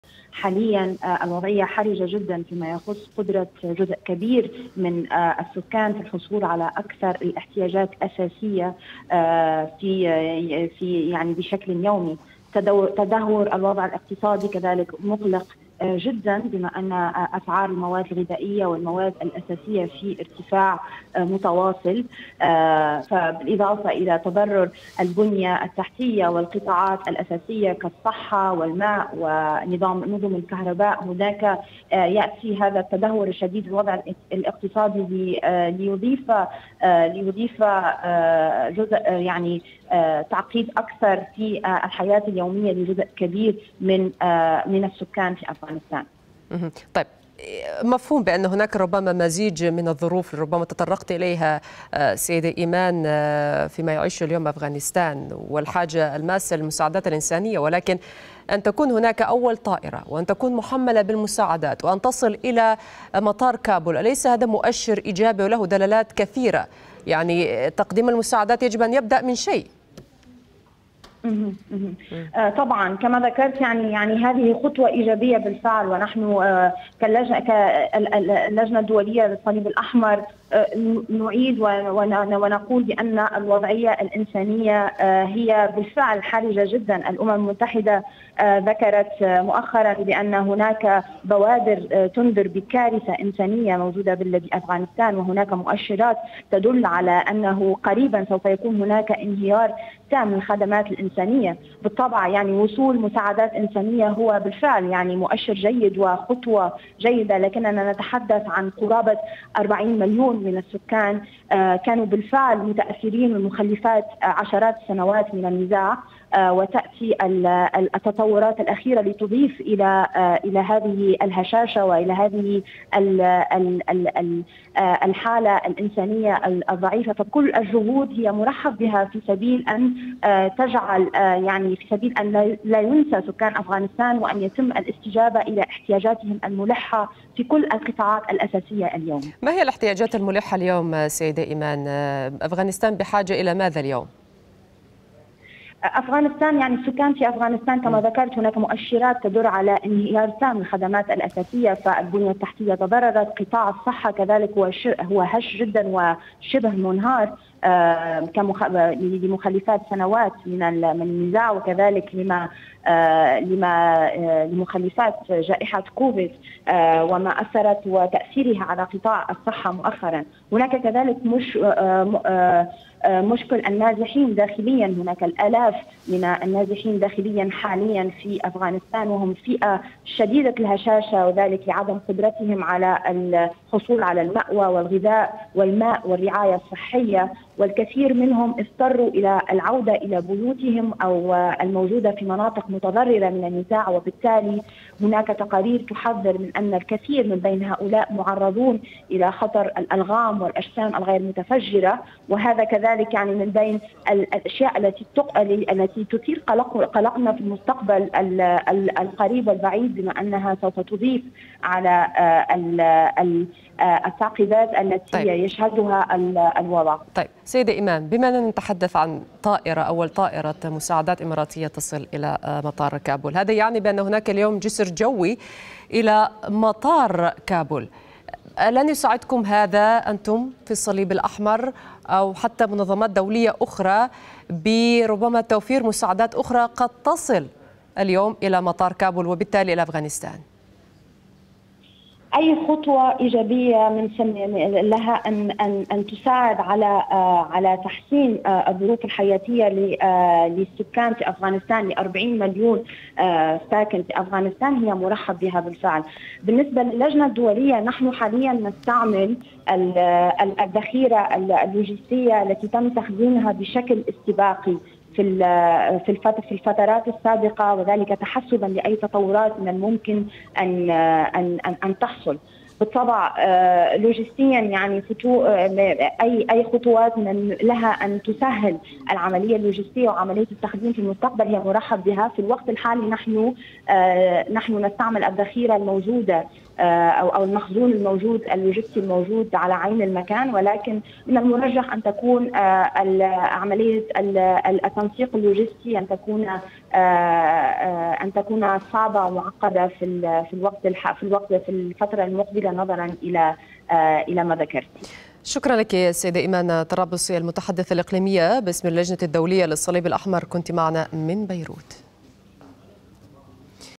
المتحدثة باسم الصليب الأحمر: هناك مؤشرات على انهيار تام للخدمات الأساسية في أفغانستان
أرسلت دولة الإمارات، الجمعة، طائرة تحمل مساعدات طبية وغذائية عاجلة إلى الشعب الأفغاني هي أول طائرة مساعدات إنسانية تصل إلى أفغانستان منذ الانسحاب الأميركي، وذلك في إطار المساهمة في توفير الاحتياجات الأساسية والضرورية لآلاف الأسر الأفغانية، سيما الفئات الأكثر ضعفاً كالنساء والأطفال وكبار السن. وفي سياق متصل قالت المتحدثة باسم الصليب الأحمر إيمان طرابلسي في مداخلة مع سكاي نيوز عربية إن هناك مؤشرات على انهيار تام للخدمات الأساسية في أفغانستان.